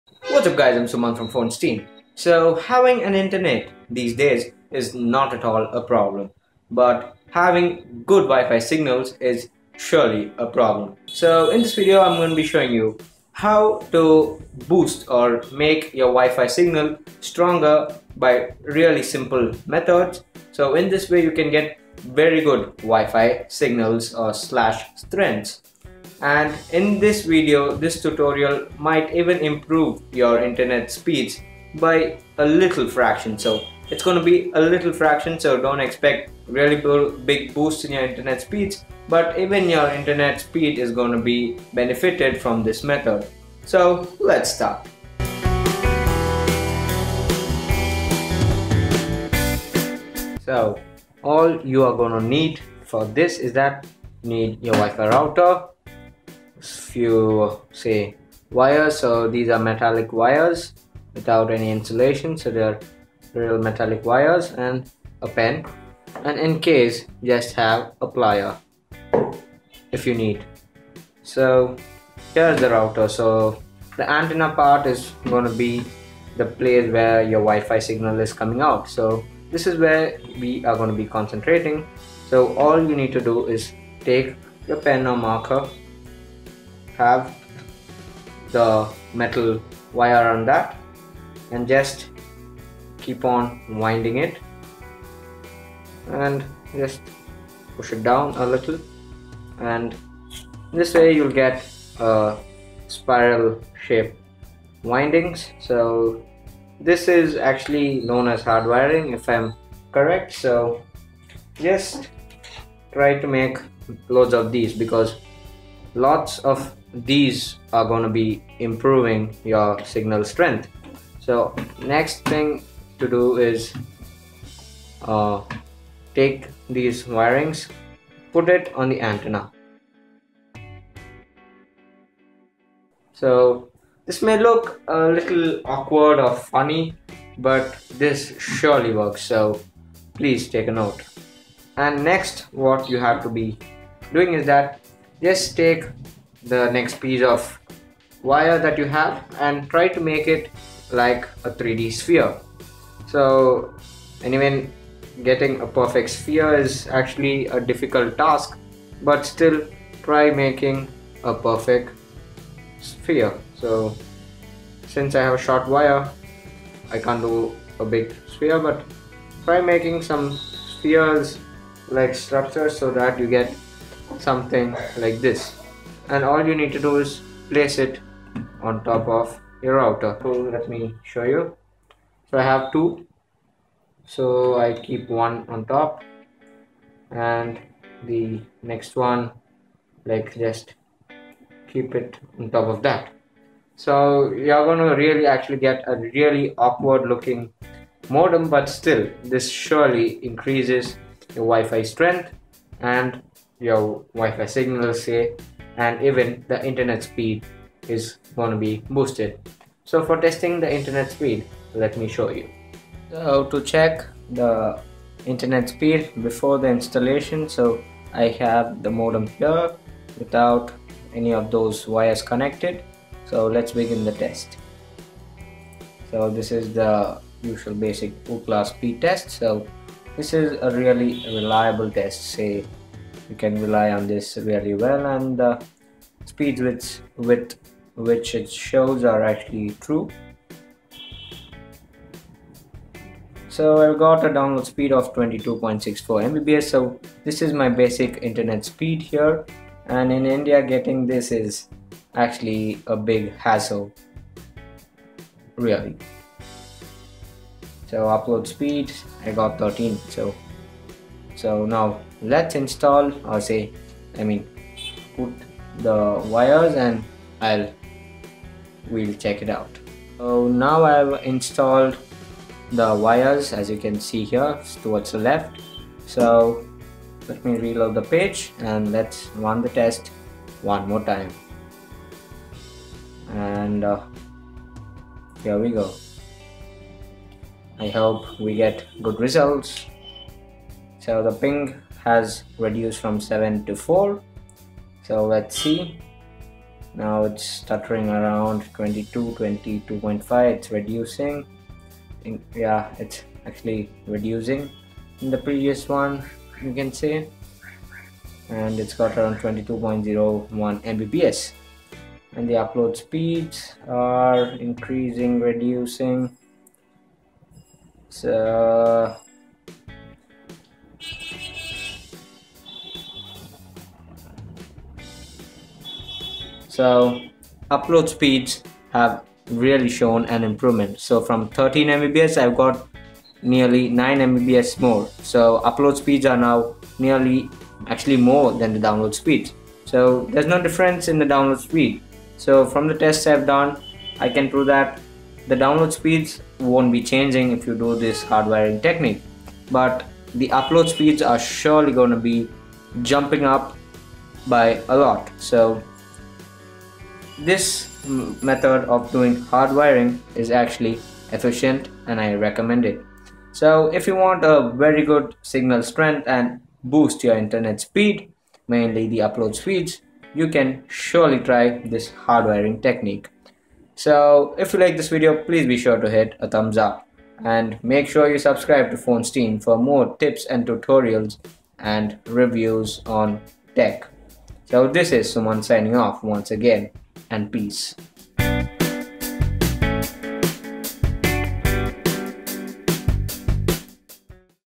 How To Increase Your Wi-Fi signal range by a simple method.100% working !!
Here I show you how to increase your internet speed and wifi signal strength only by a simple method . It worked 100% fine for me . Let me know  if it worked for you by leaving a comment in the description.